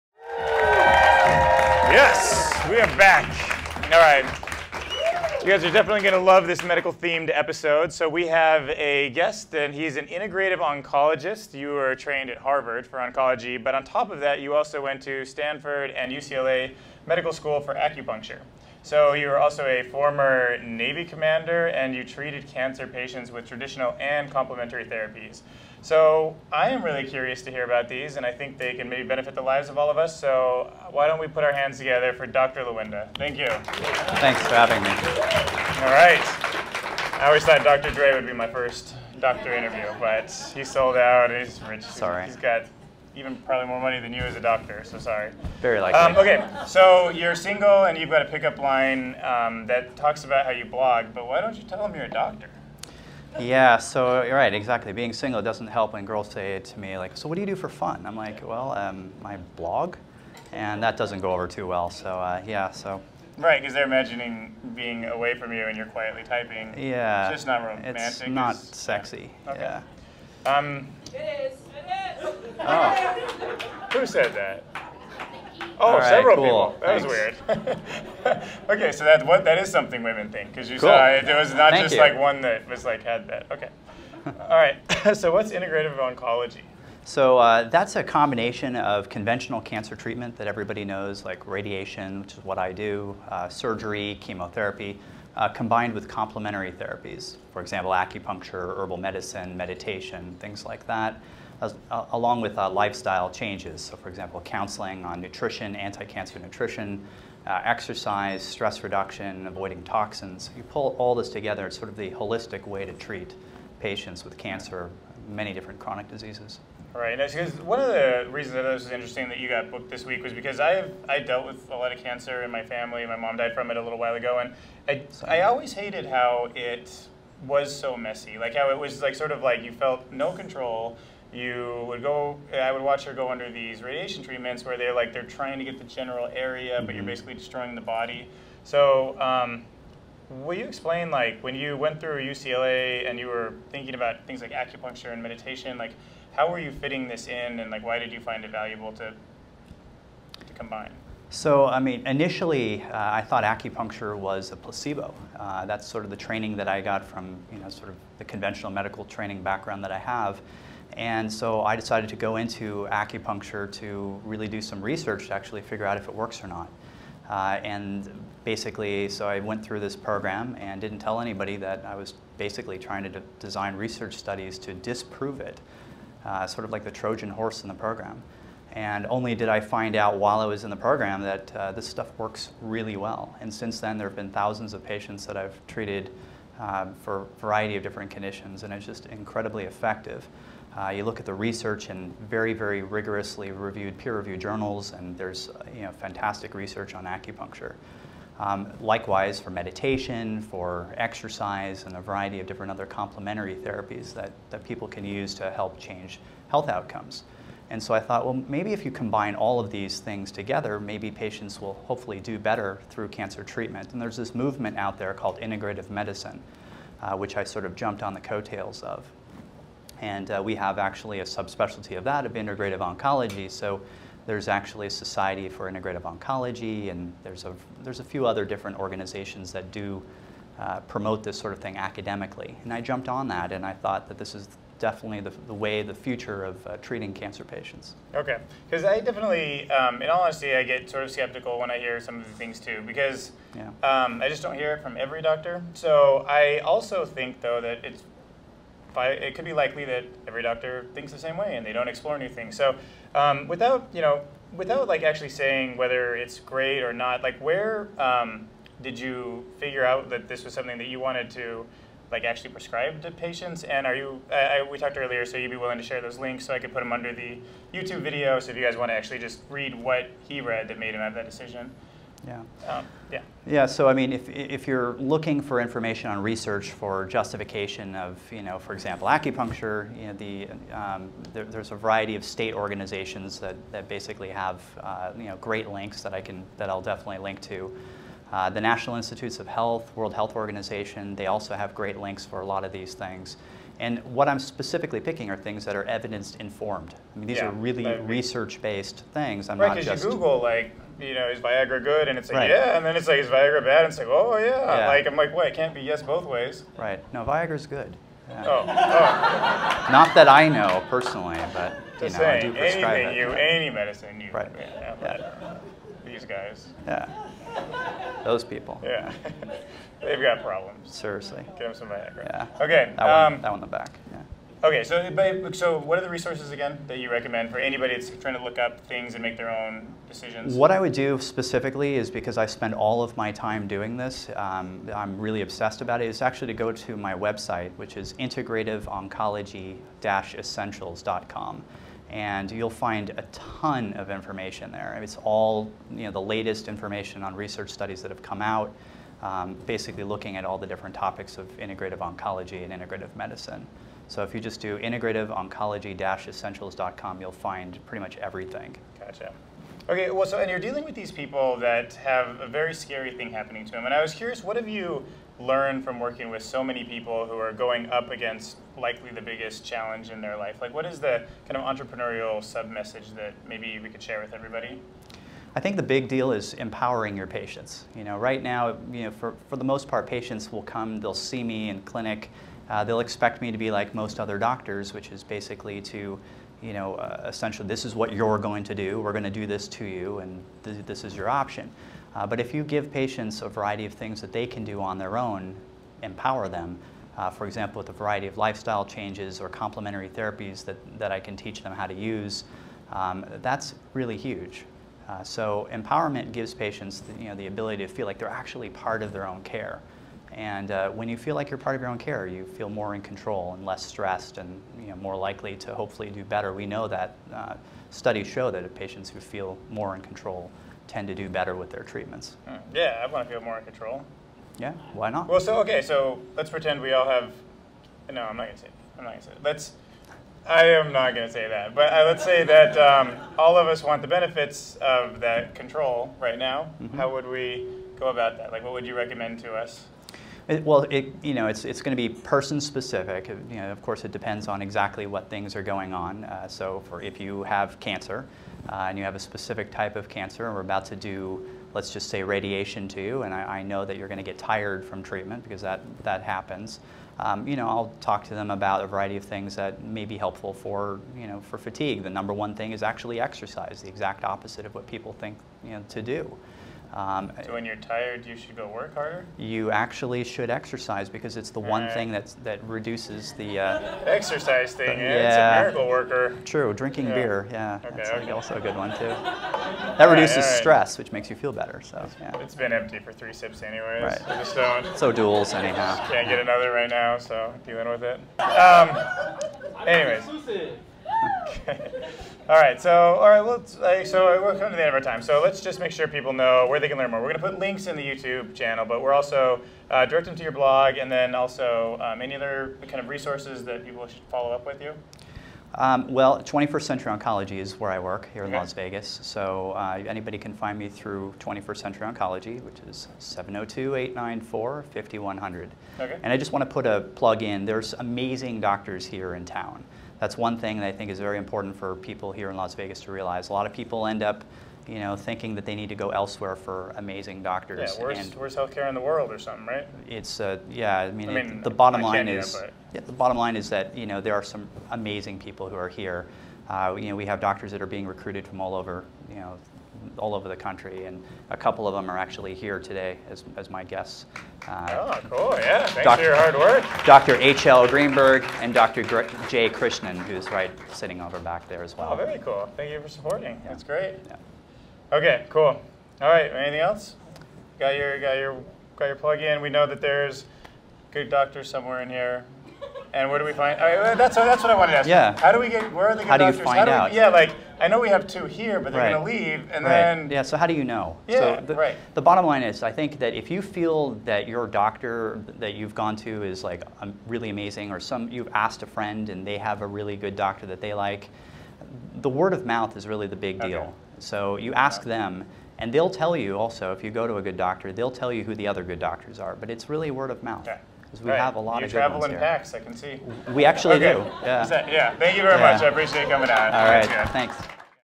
Dr. Brian Lawenda
Ep 83 - Downtown Podcast (ft. Dr. Brian Lawenda)

Description:
Gear up for episode #82 of the Downtown Podcast! In this episode, we feature integrative oncologist Dr. Brian Lawenda as he talks about preventative measures against cancer.

We also speak to Kelly Fairchild with Las Vegas Walking Tours and Charles Ressler tells us all about the Twitter hashtag helping dreams come true.

We thank our Episode Sponsor Accion Nevada for helping local entrepreneurs fund their dreams!

Join our mailing list at downtownpodcast.tv to see our bonus interview with Tara Fall.